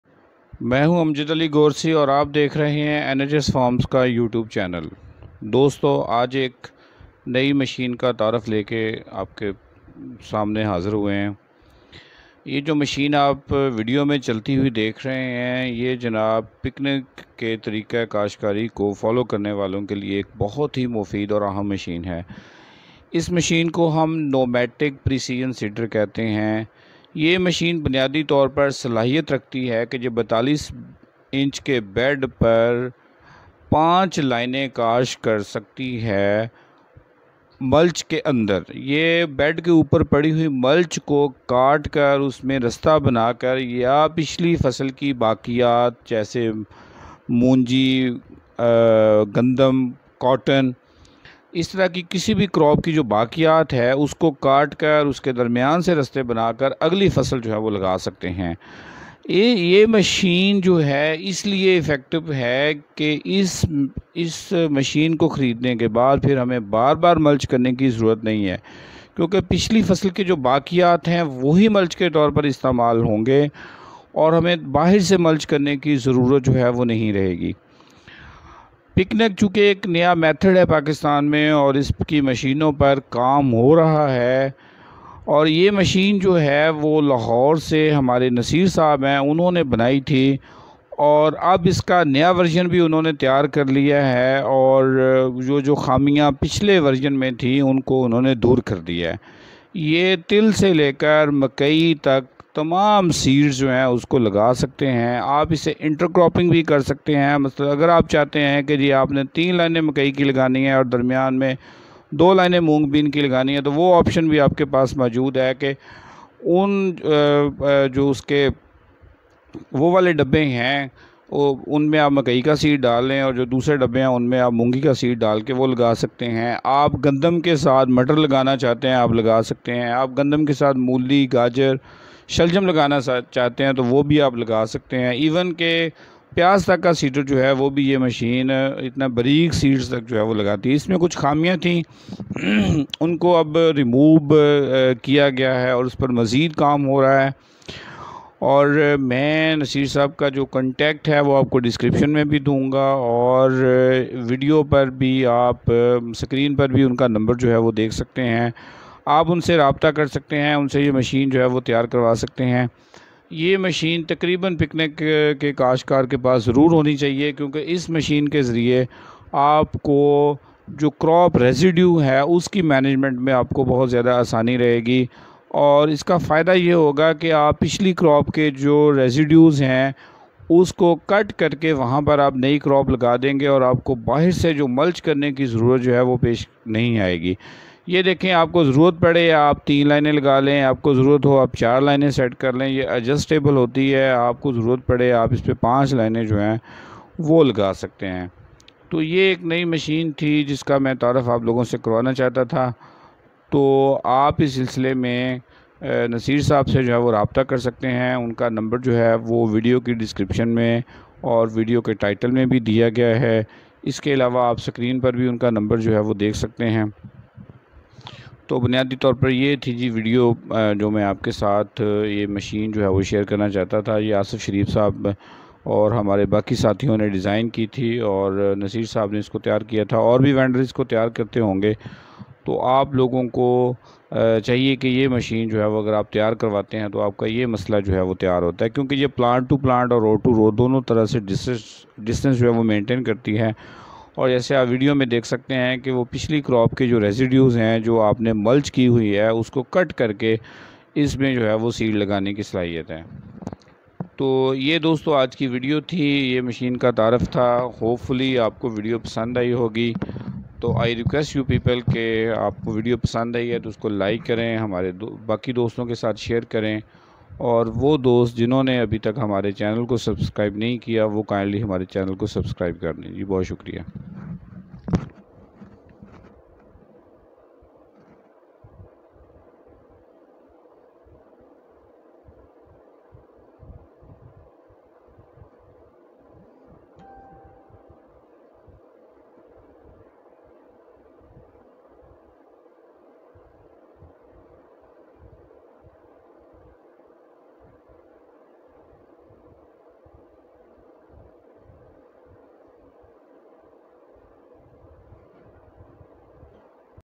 No- Till Precision Seeder for seeding on 42 Inches beds. ملچ کے اندر سیڈ لگانے والی مشین
پقنک کاشتکاروں کے لیے مشینوں کی عدم دستیابی ایک بڑا مسلہء ہے۔ اس سلسلے میں بہت سی کوششیں کی جا رہی ہیں کہ اسطرح کی مشینیں بنائی جائیں جو لائیو یا ڈیڈ ملچ کے اندر ہر سائز کے سیڈز لگا سکیں۔۔ اسی طرح کی مشین کا تعارف اس ویڈیو میں دیا گیا ہے۔ مشین کی تیاری کے لیے مندرجہ ذیل نمبروں پر رابطہ کیا جاسکتا ہے۔
محمد نصیر صاحب ( فارم مور  لاہور والے) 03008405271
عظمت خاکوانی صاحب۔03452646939